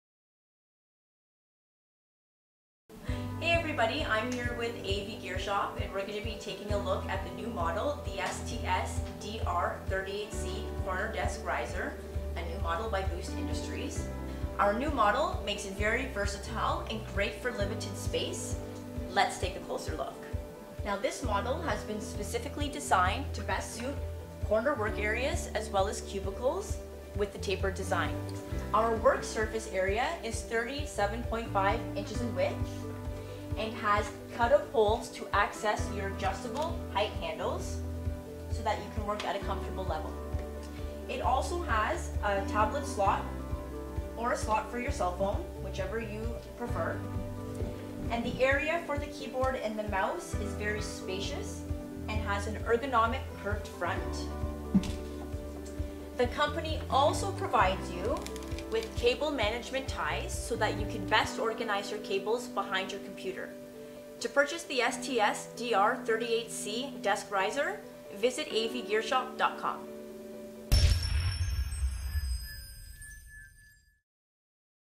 Boost Industries STS-DR38C Corner/Cubicle Sit-to-Stand Desk Riser
Whether you have a corner workspace or a cubicle, you too can do your work sitting or standing. The Boost Industries STS-DR38C desk riser easily fits into corners and is the perfect sit/stand desk for cubicles.

More Ways To Follow AVGearshop